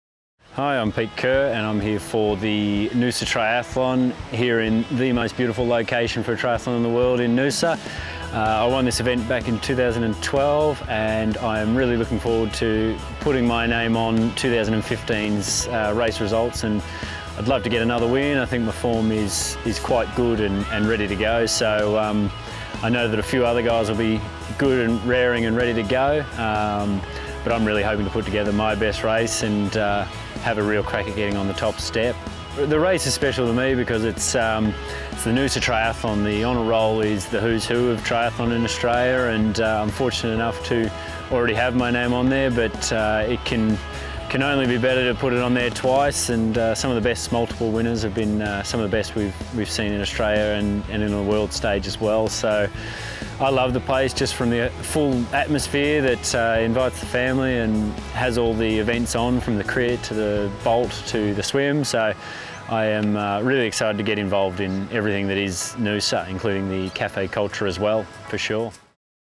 Noosa Triathlon with Peter Kerr
Peter Kerr holds his 2012 Noosa Triathlon victory close to his hear. Hear what the previous champion has to say as he heads into the 2015 race.